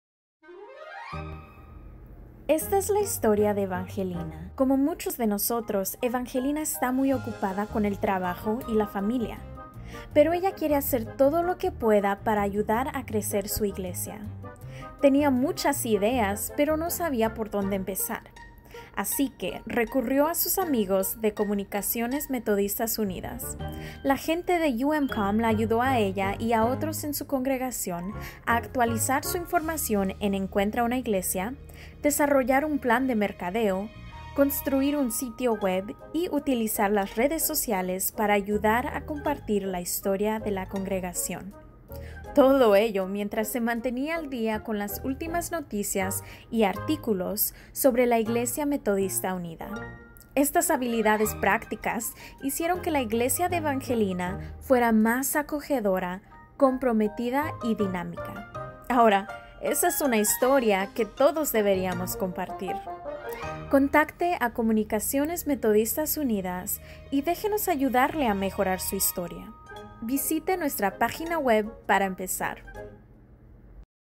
La historia de Evangelina
Vea este video para ver cómo UMCOM ayudó a la iglesia de Evangeline a desarrollar una estrategia de comunicaciones y mercadeo.

Este canal es administrado por Comunicaciones Metodistas Unidas en Nashville, TN, EEUU.